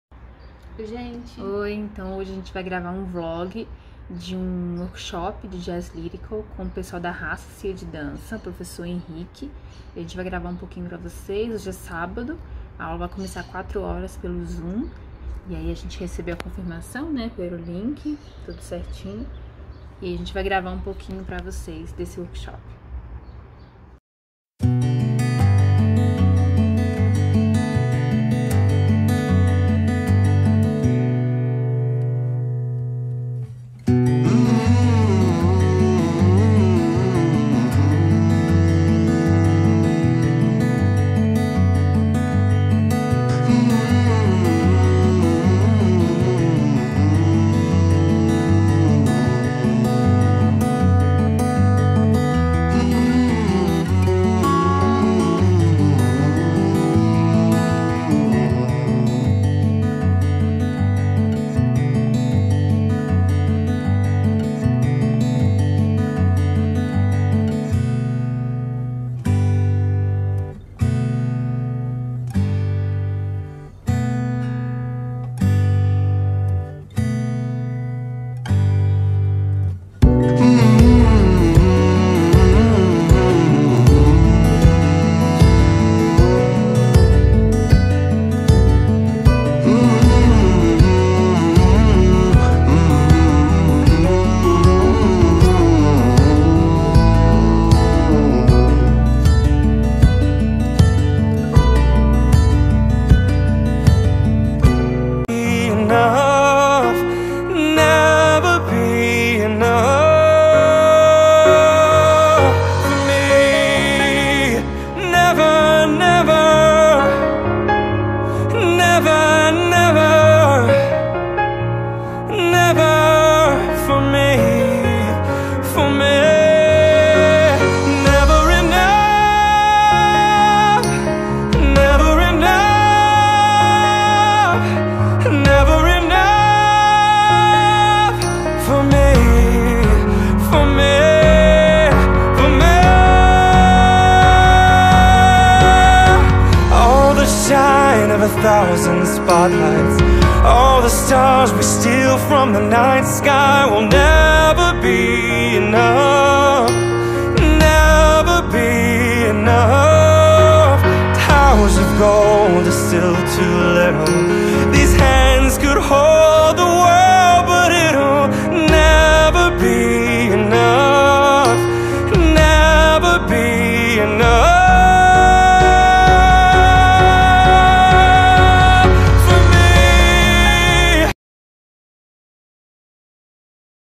Vlog- aula de jazz lyrical
Vlog de uma aula( online) de jazz oferecida pelo Grupo Raça